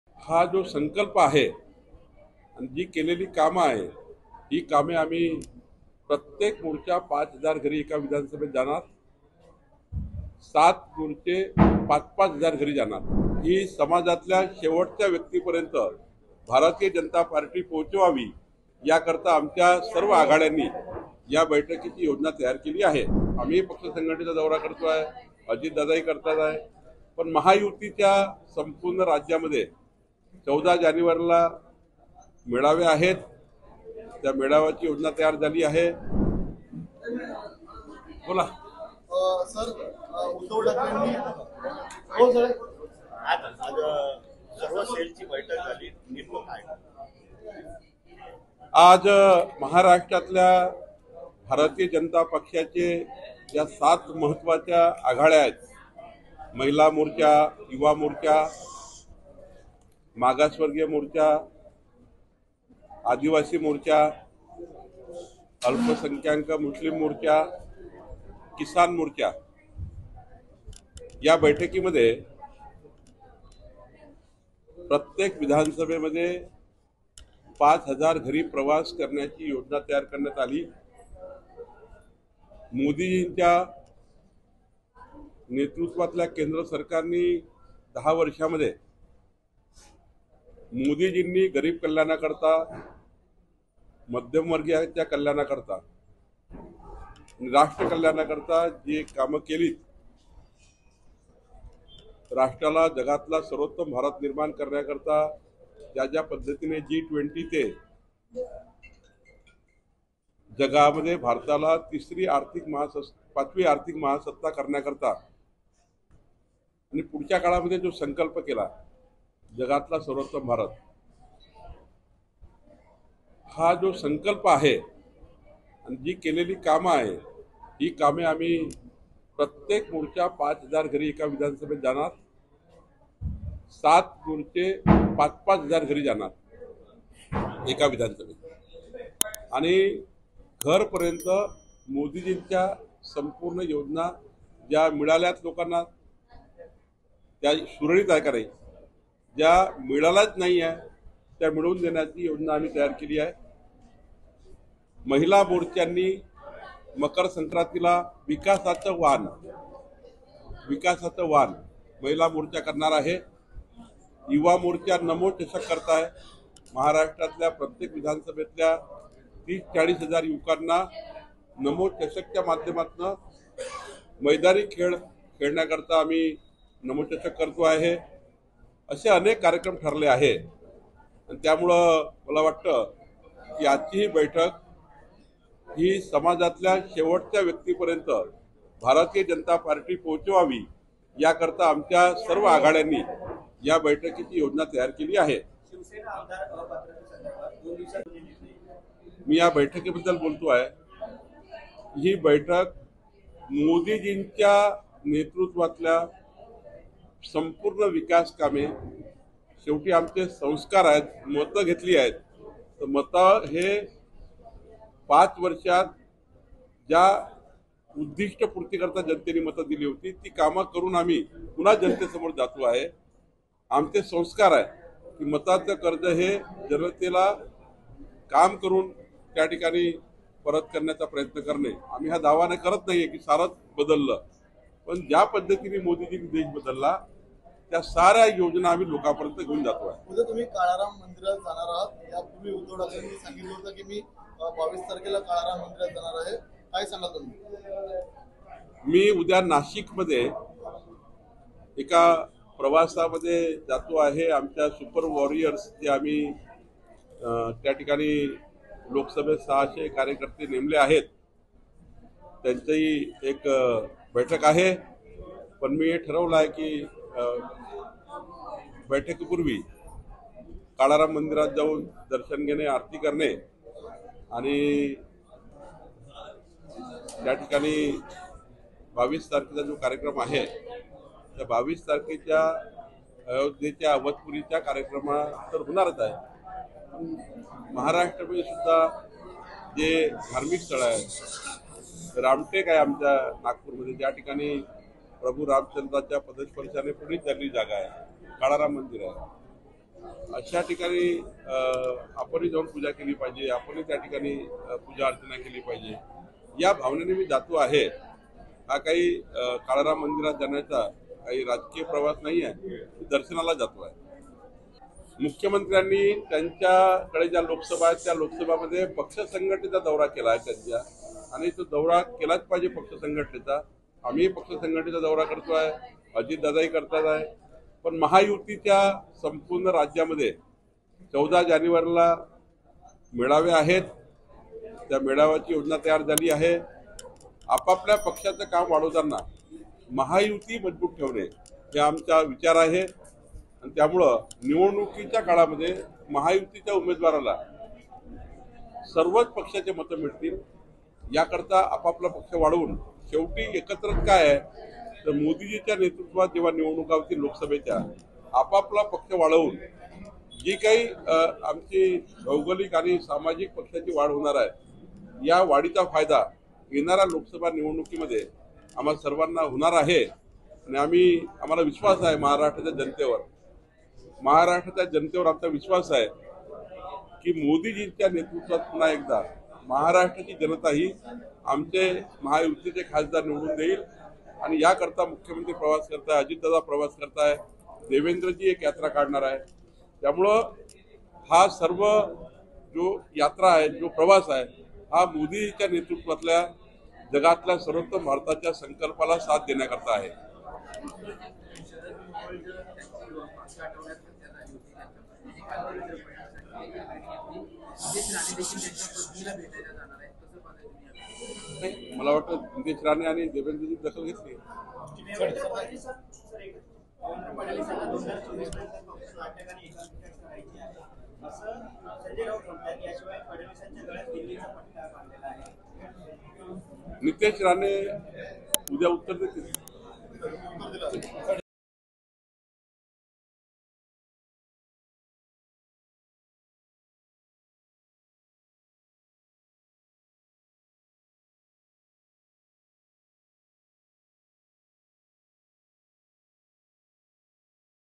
Chandrashekhar Bawankule : महायुतीचा लोकसभेचा प्लॅन ठरला, बावनकुळेंनी सांगून टाकला... | LetsUpp
महायुती आगामी लोकसभा निवडणुकीला कशा पद्धतीनं सामोरं जाणार आहे? याबद्दल भाजपचे प्रदेशाध्यक्ष चंद्रशेखर बावनकुळे यांनी जाहीरपणे सांगितले आहे. यावेळी बावनकुळे नेमकं काय म्हणाले? पाहा